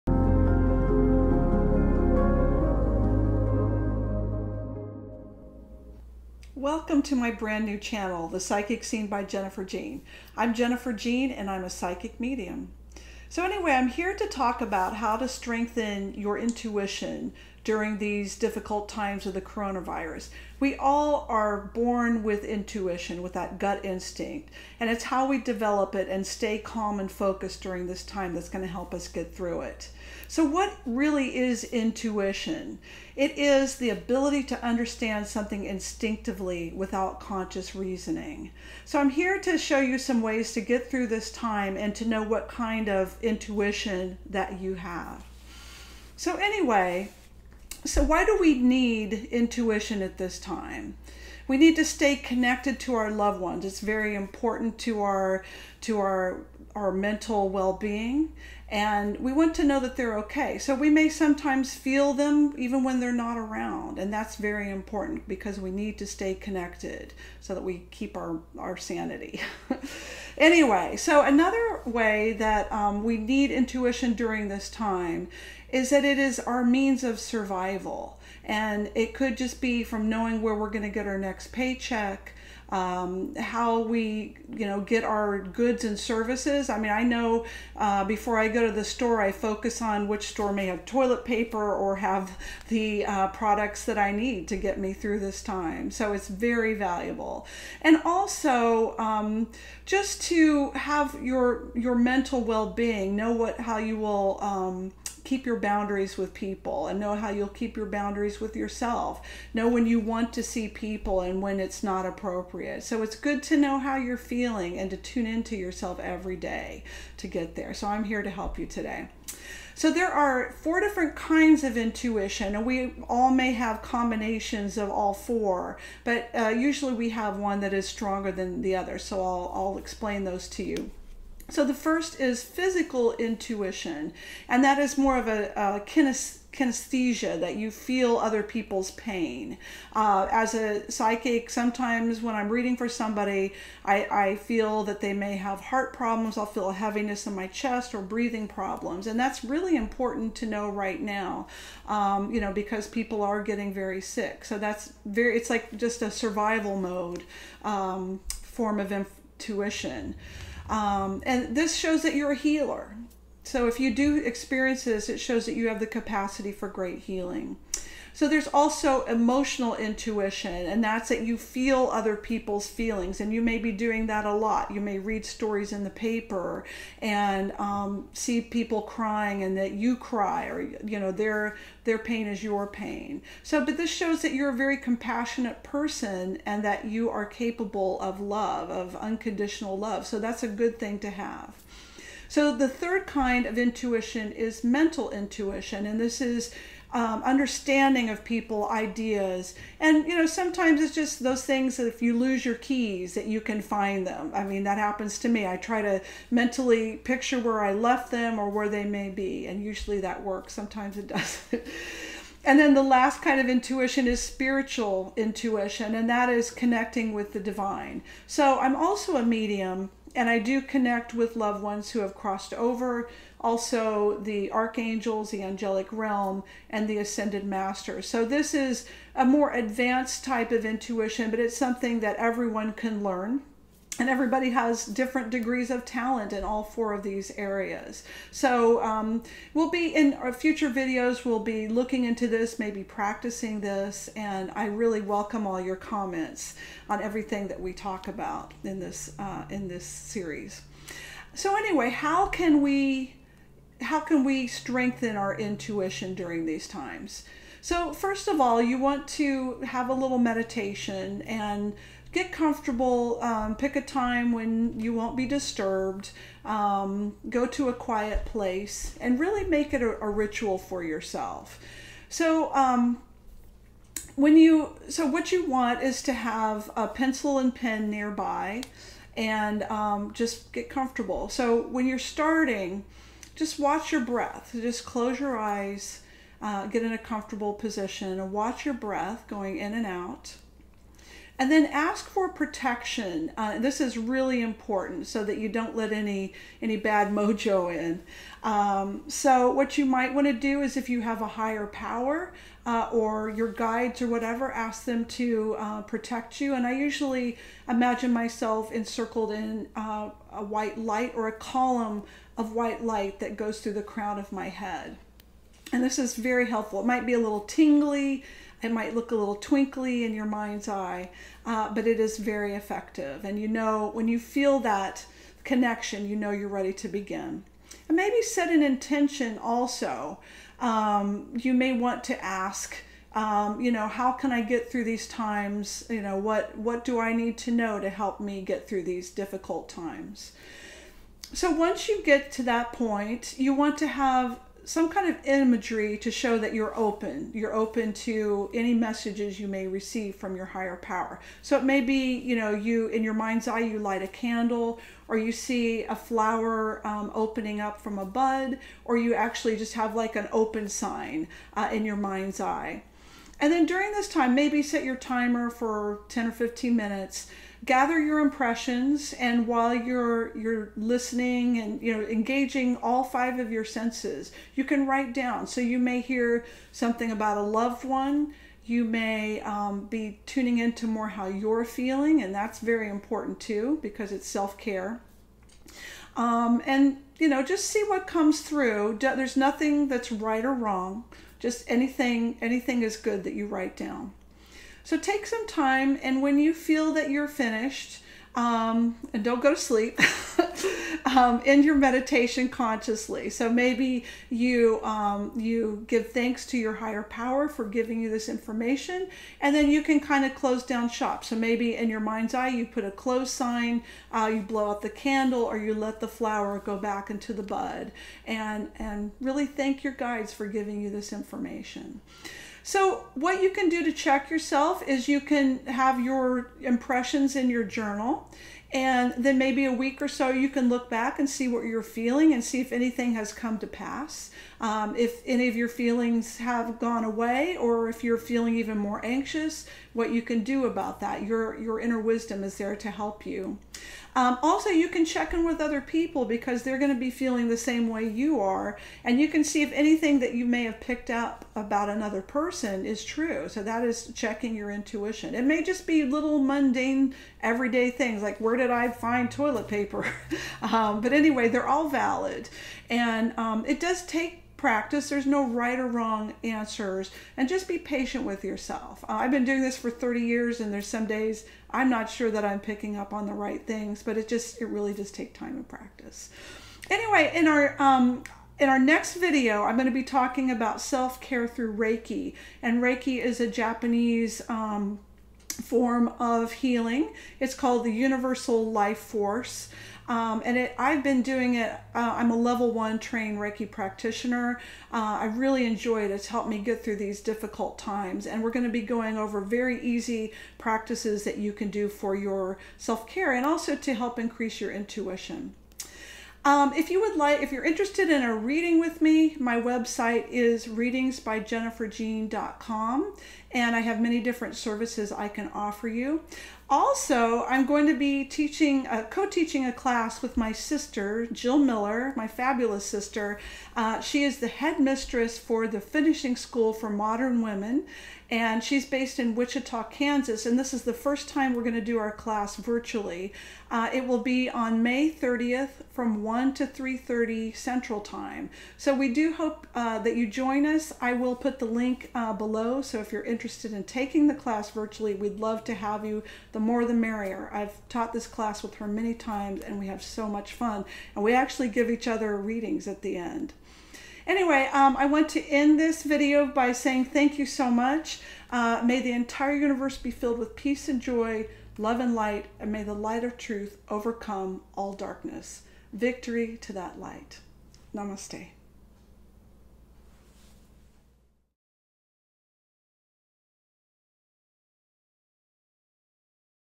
How to strengthen everyday intuition in the times of Coronavirus
Psychic Medium Jennifer Jean will guide you through reasons why it’s important to hone your intuition during these difficult times and practical tips to connect to your higher self.

Information for Everyday Intuition virtual class on May 30, 2020

Four kinds of intuition:
1. Physical - feels physical sensations - energy healer
2. Emotional - feels others emotions - compassionate healer
3. Mental - receives information - wisdom and knowledge
4. Spiritual - connection to the divine - mediumship and spirit realm